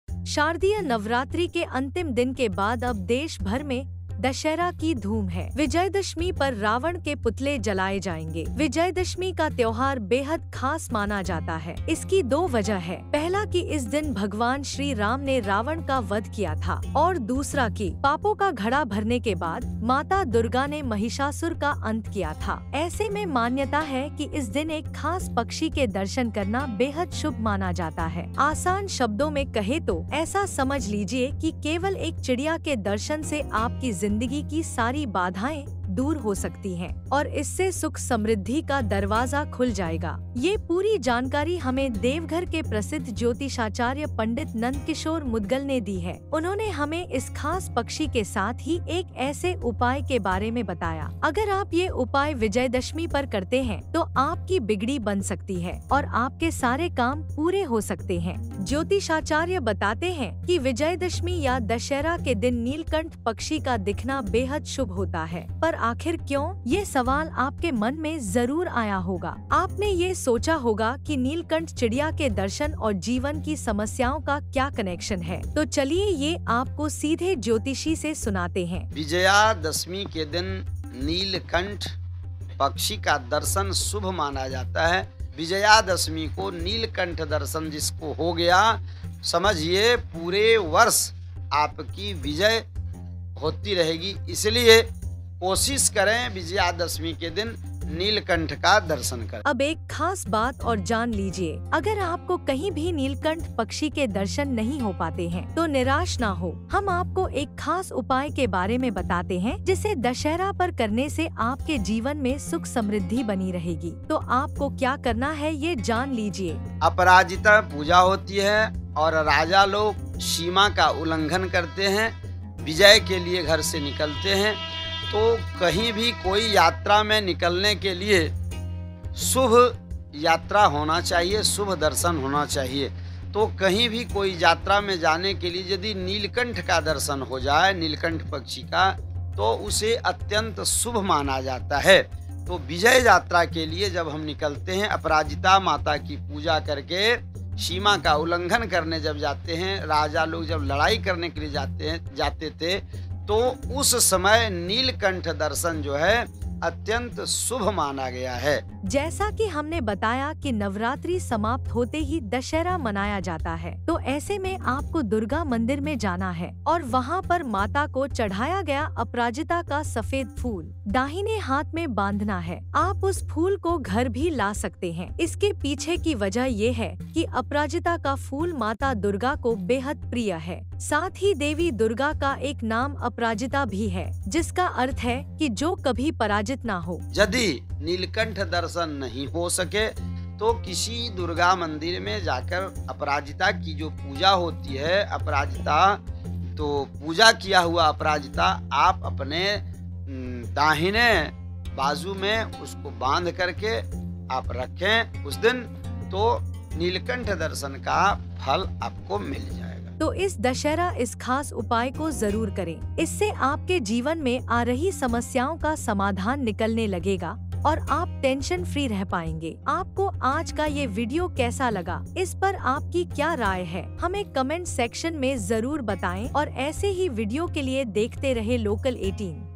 Dussehra पर करें Neelkanth के दर्शन, पाएं सभी बाधाओं से मुक्ति!
After the Shardiya Navratri, the festival of Vijayadashami is being celebrated across the country with great enthusiasm. In this special occasion, learn why sighting the Nilkanth bird is considered auspicious and how the white flower of Aparajita can bring happiness and prosperity into your life. Get insights from astrologer Pandit Nandkishore Mudgal about the significance of this festival and how this special remedy can help resolve your issues. This Vijayadashami, by observing the Nilkanth and practicing this remedy, you can invite positivity into your life.
शारदीय नवरात्रि के बाद देशभर में विजयदशमी का पर्व धूमधाम से मनाया जा रहा है। इस खास अवसर पर जानिए क्यों नीलकंठ पक्षी के दर्शन करना शुभ माना जाता है और कैसे अपराजिता के सफेद फूल का उपाय आपके जीवन में सुख और समृद्धि ला सकता है। ज्योतिषाचार्य पंडित नंदकिशोर मुद्गल से जानें इस पर्व की खासियत और यह विशेष उपाय कैसे आपकी समस्याओं का समाधान कर सकता है। इस दशहरा, नीलकंठ के दर्शन और उपाय को अपनाकर आप अपने जीवन में सकारात्मकता ला सकते हैं। वीडियो देखकर हमें अपनी राय कमेंट सेक्शन में बताना न भूलें।